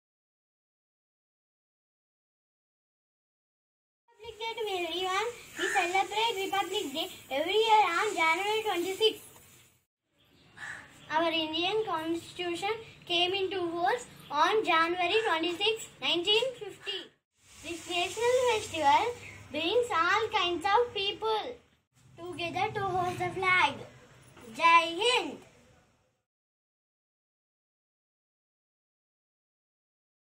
Happy Republic Day 👬👭
Happy 72 Republic Day guys. hereis a video about Republic Day from our special one. believing that 'today's children are tomorrow's citizen'